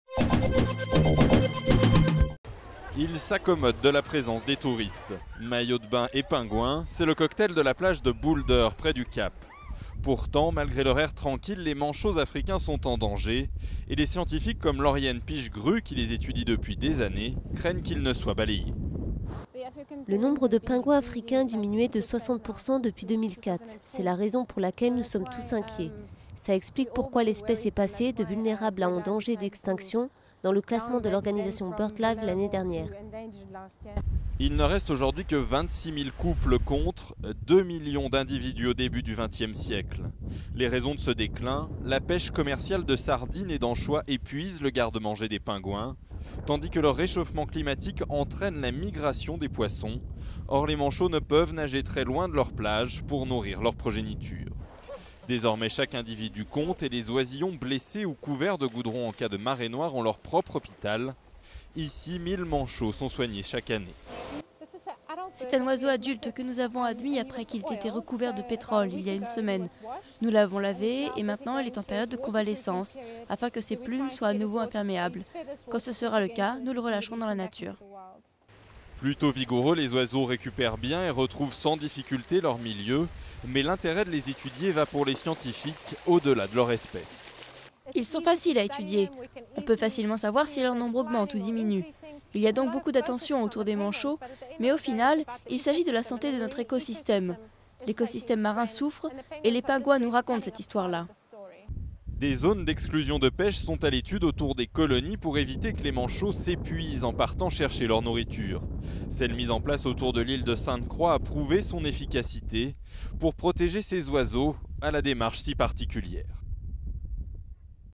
Au Cap, les manchots comme indicateurs de la santé des mers
Les manchots font partie des attraits touristiques de l'Afrique du Sud. Protégés dans la réserve de Simon's Town, cette espèce africaine est pourtant menacée : le nombre d'individus a diminué de 60% depuis 2004. Un nouveau signe de la dégradation du milieu marin. Durée: 01:57